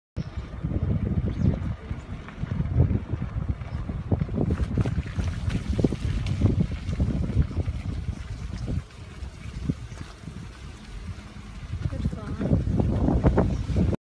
kruger national reserve
we like it so much to see all this beautiful wild animals